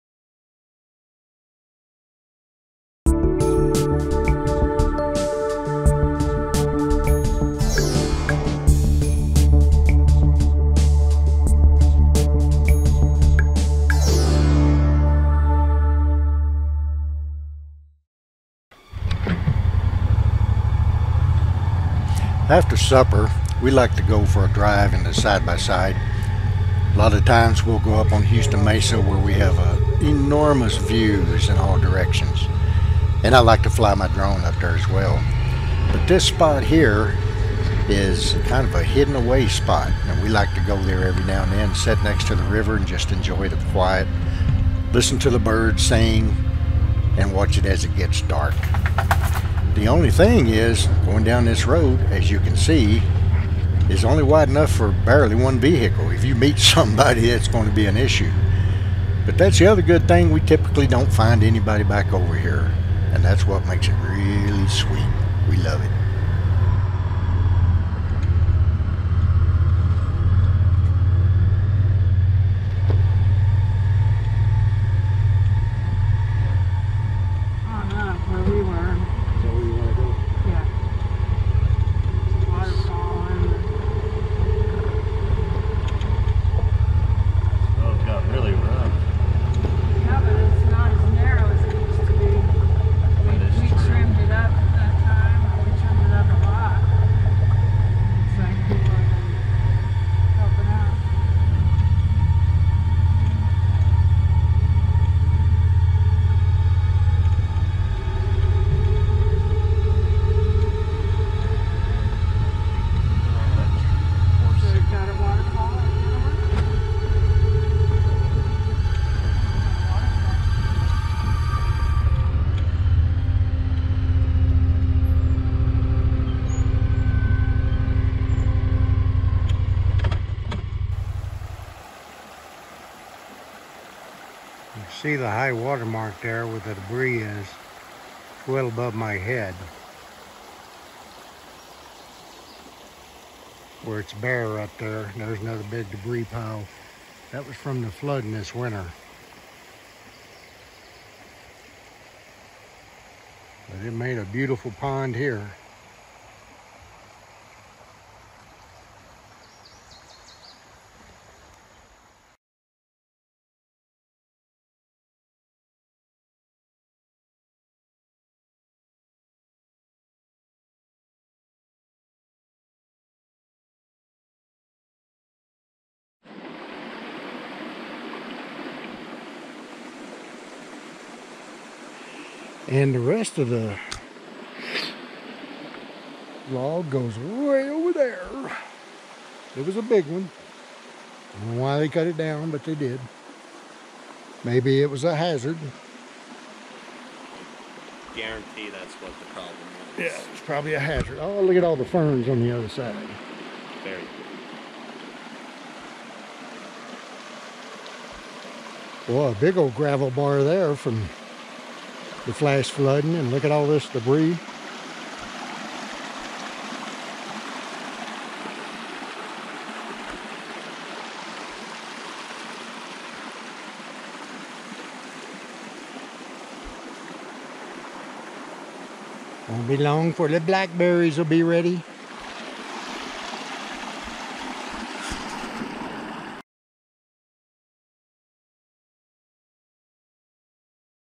A day with Rand
Here is a short video of a couple of our favorite local getaways during the week.